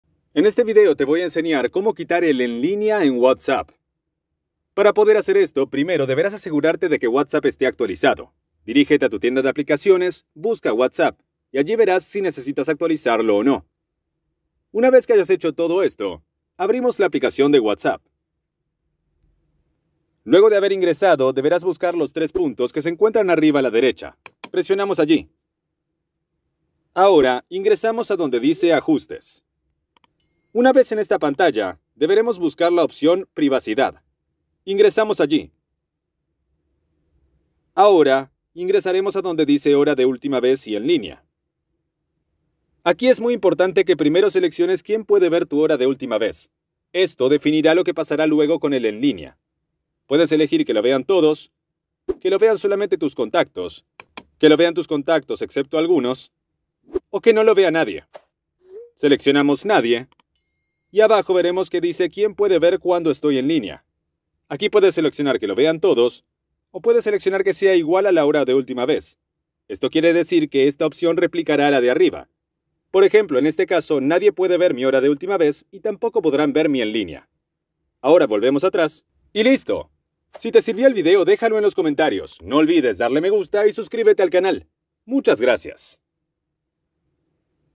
Como quitar el en línea en whatsapp 2024 (GUÍA COMPLETA)
Aprenderás como quitar el en línea en whatsapp 2024. Sirve para como quitar el en línea en whatsapp iphone/android. Básicamente, como quitar en linea de whatsapp 2024 o como desactivar el en línea en whatsapp.